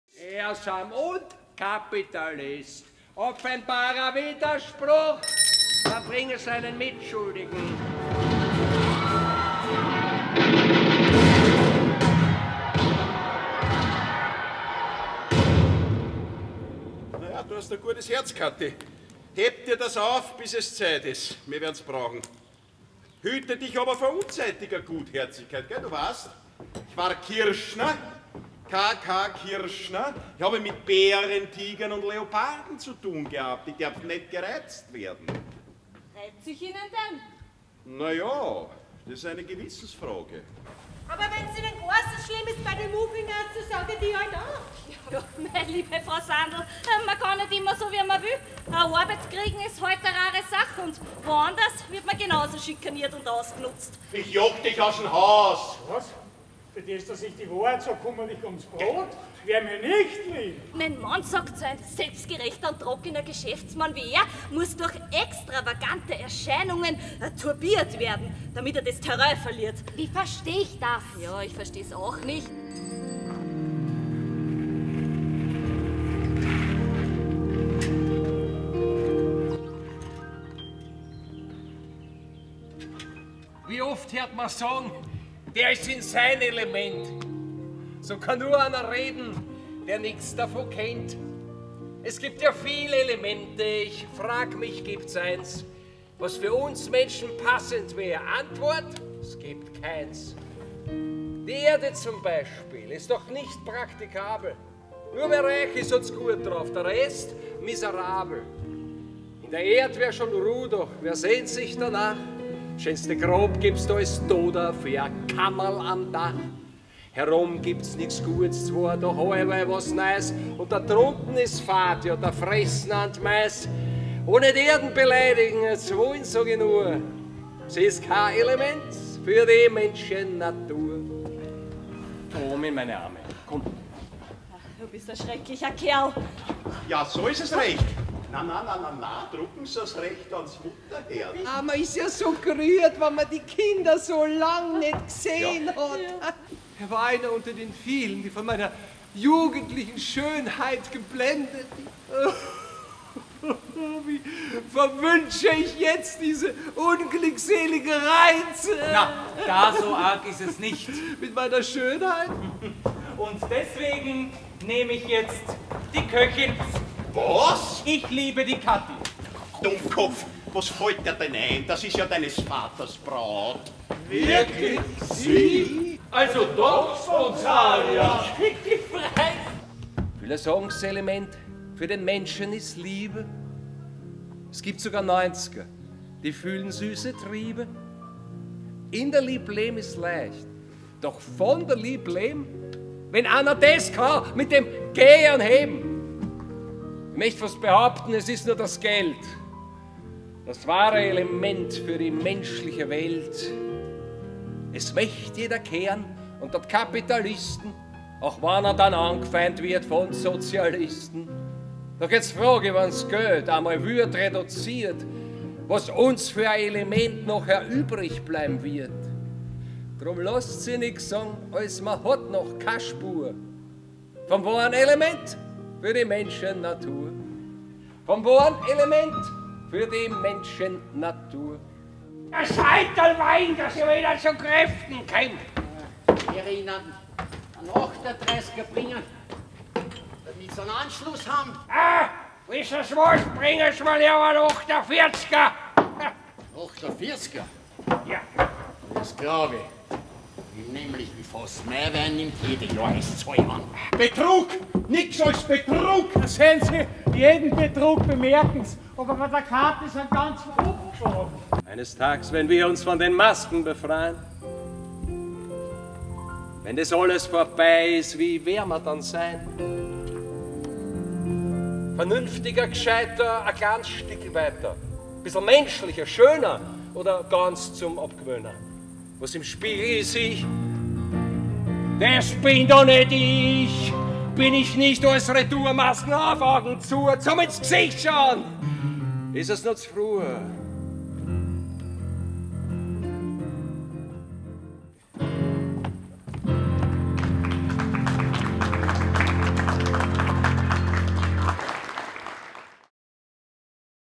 TRAILER: 49. NESTROY Spiele Schwechat | CHARIVARI | 2021
Ein Mann, gescheitertes Überbleibsel der revoltierenden 1848er-Generation, völlig mittellos und ohne jegliche Zukunftsperspektive, bekämpft in einer Art Privatrachefeldzug einen skrupellosen Repräsentanten des selbstgerechten, neureichen Großbürgertums. Er entlarvt dessen Doppelmoral, deckt seine finanziellen Machenschaften auf und wird schlussendlich zu seinem Albtraum, indem er sich in irrwitzige Karikaturen und bizarre Gestalten verwandelt. Die Wiederentdeckung und Rehabilitierung eines völlig vergessenen schrägen Verwirrspiels, das alle Elemente der Nestroy´schen Komödienkunst enthält.

mit dem Nestroy-Ensemble rund um Oliver Baier, Michelle Haydn, Rainer Doppler und  Ines Chihal
Intendanz: Peter Gruber

Video & Schnitt: Bernadette Dewald